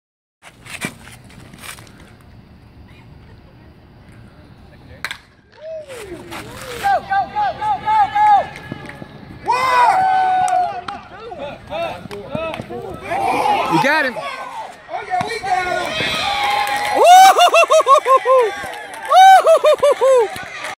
to SPENCERUNRUH for ThrowOut @ Home
Relay from OF to Infield for the Out @ the Plate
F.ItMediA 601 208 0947